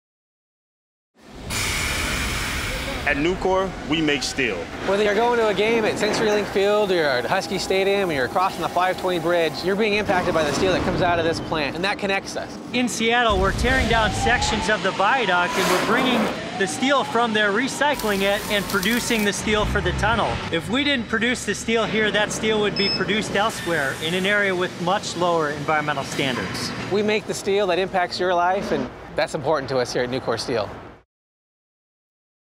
AWB "Grow Here" - Nucor Steel
AWB is Washington State's Chamber of Commerce. This TV commercial featuring Seattle's Nucor Steel is part of AWB's "Grow Here" advertising campaign, which promotes the state's businesses and their environmental practices. AWB and their agency Daniels Brown Communications worked with BigHouse to create TV spots, web films and radio commercials to tell these stories.